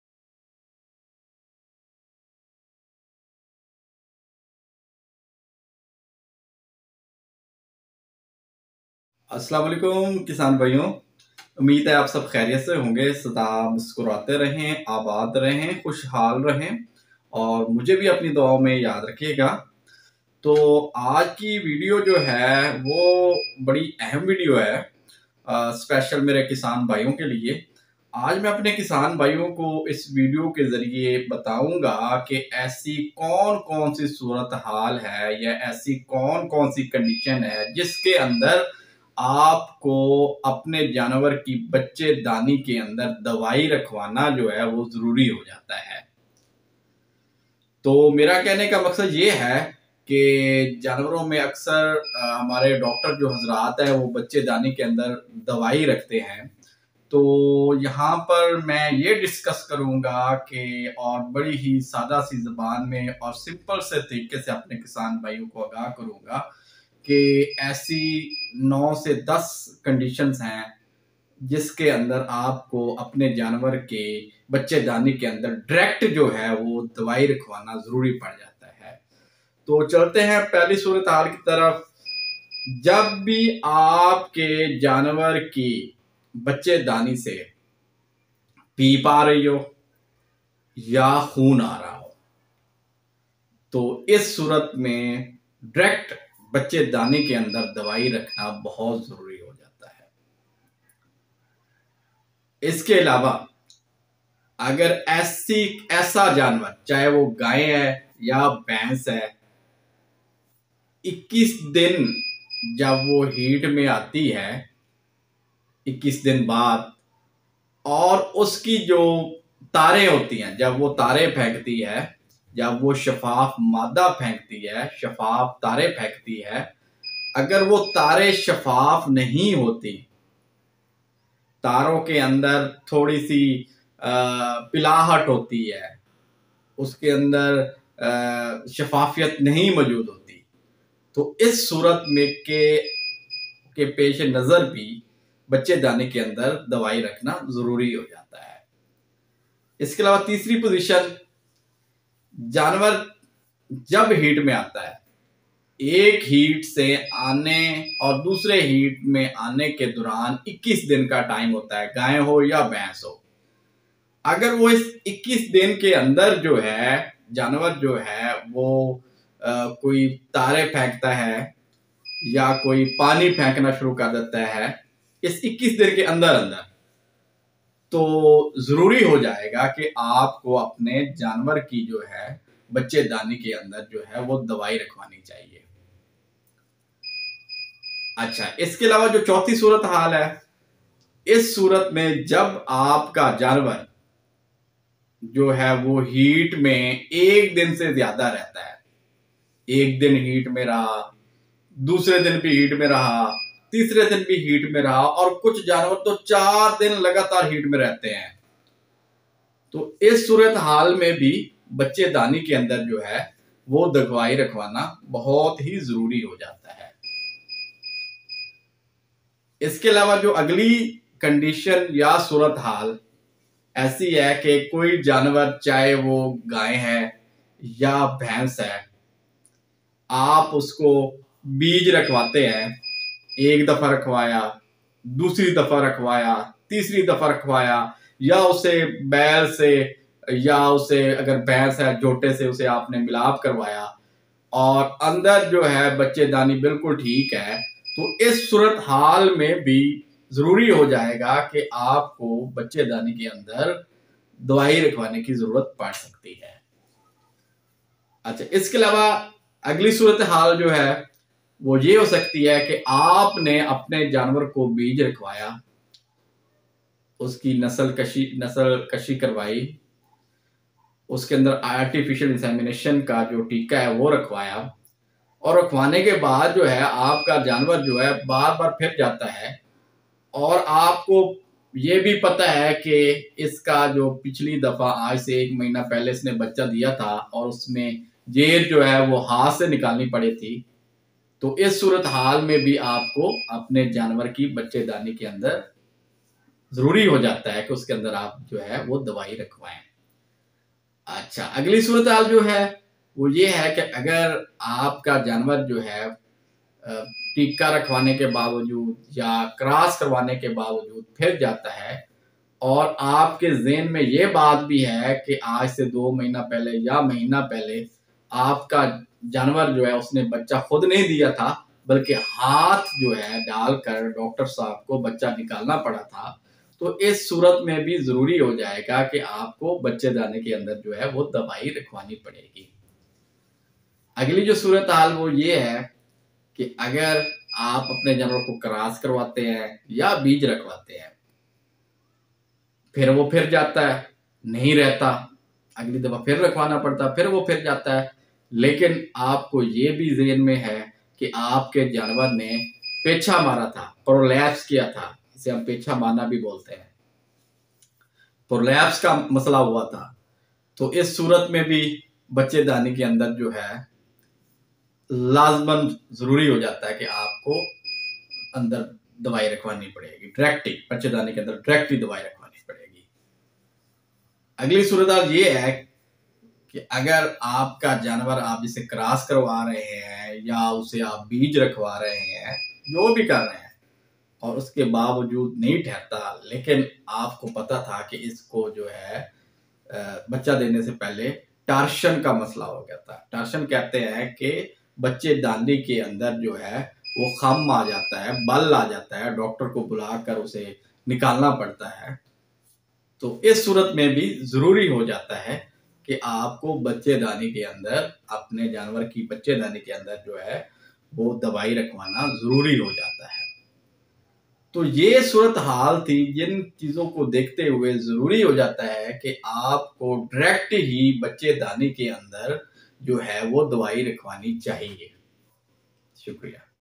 Treatment of Uterine infection in Cattle |Antibiotics for Uterine infection in Buffalo and cow
گاۓ اور بھینس کی بچے دانی ( Uterus) کے اندر کس صورت میں یا کس بیماری میں دوائی رکھنا ضروری ہو جاتا ہے